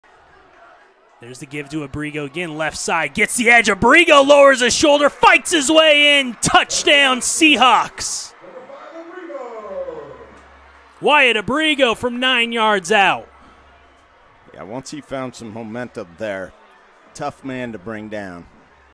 Wyatt Abrigo 9-Yard TD
Wyatt Abrigo punches in the first TD of the Fish Bowl to give Peninsula a 7-0 lead late in the first half.